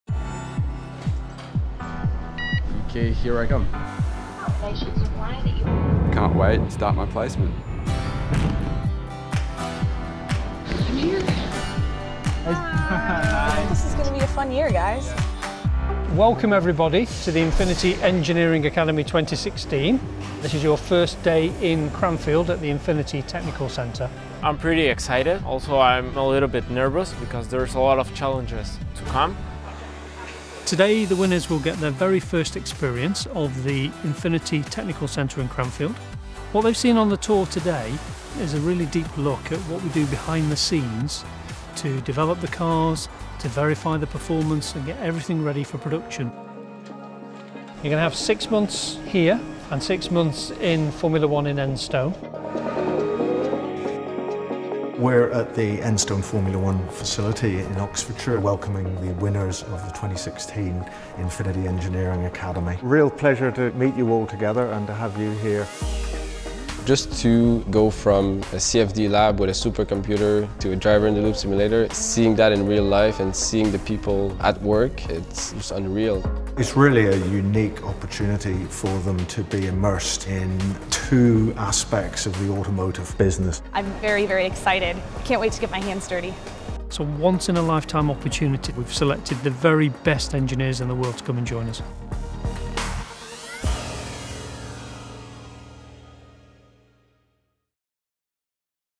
INFINITI Engineering Academy 2016 - Welcome to the Academy
لقد بدأ فائزو أكاديمية إنفينيتي للهندسة السبع في نوفمبر سنة التنسيب مع كلا من إنفينيتي وفريق رينو سبورت للفورمولا ١. كانت أيام أولى حافلة حيث بدأ الفائزون بالتأقلم في الأكاديمية.

The seven INFINITI Engineering Academy Winners commenced their placements with both INFINITI and Renault Sport Formula OneTM Team in November. They had a busy first few days of their placement, as they settled into life in the Academy